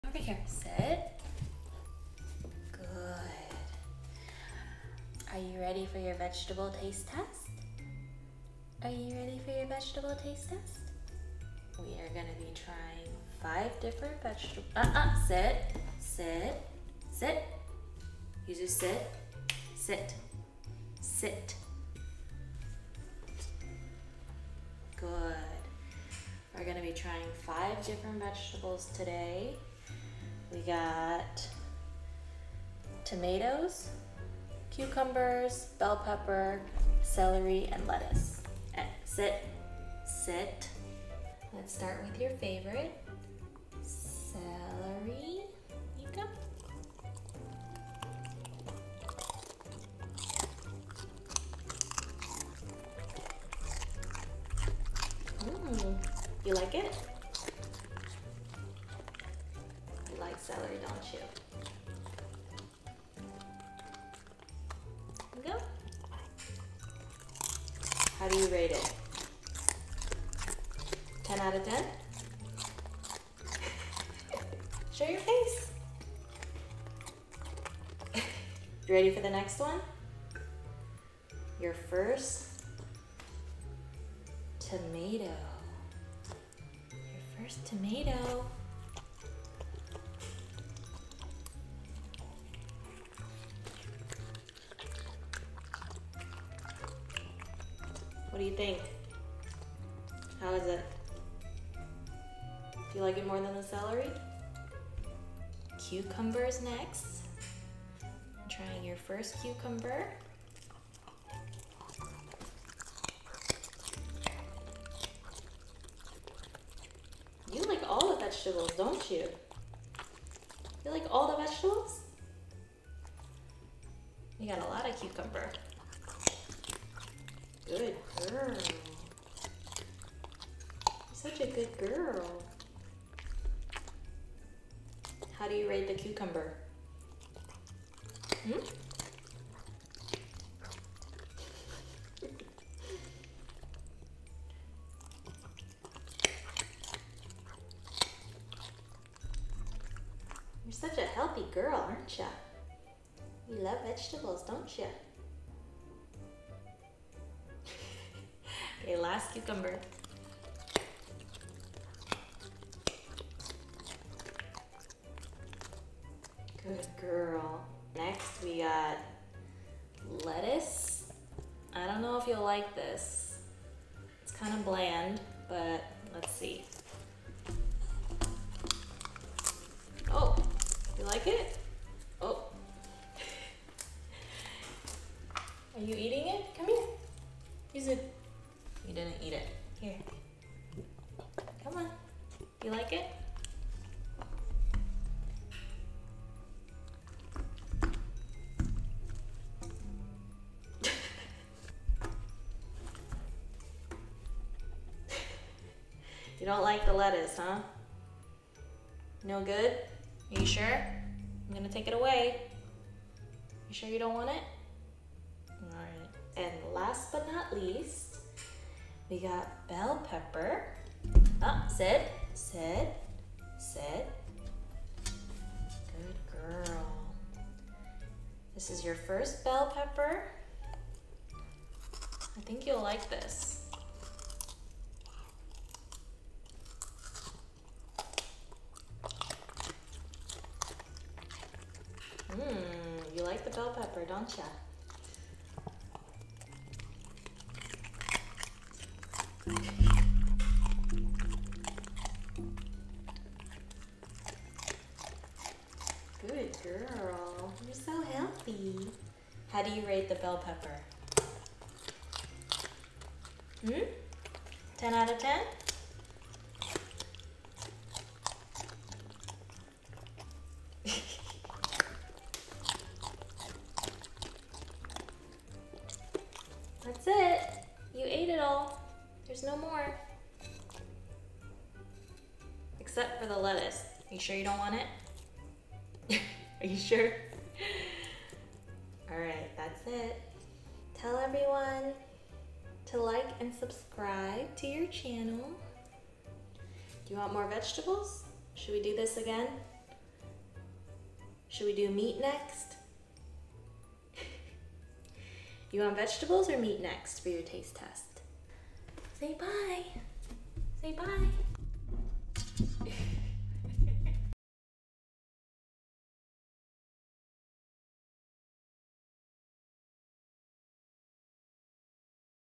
【ASMR】Funny Golden Retriever Puppy Tries Vegetables For The First Time | Yuzu Tails Taste Test
FOLLOW ME ▹